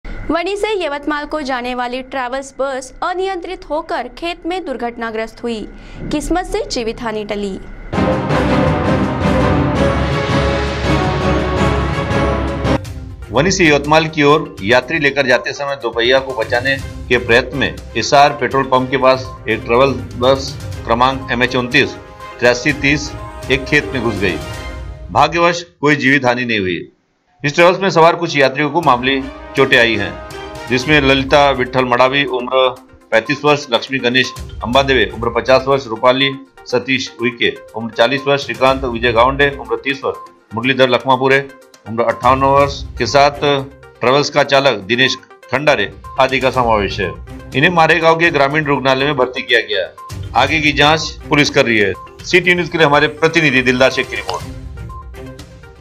CTV News 23 July यात्रीयो से भरी ट्रॅव्हल्स बस नियंत्रण खोने से खेत मे अपघात ग्रस्त हुई
वणी से यवतमाल जाते समय यात्रीयो से भरी ट्रॅव्हल्स बस नियंत्रण खोने से खेत मे अपघात ग्रस्त हुई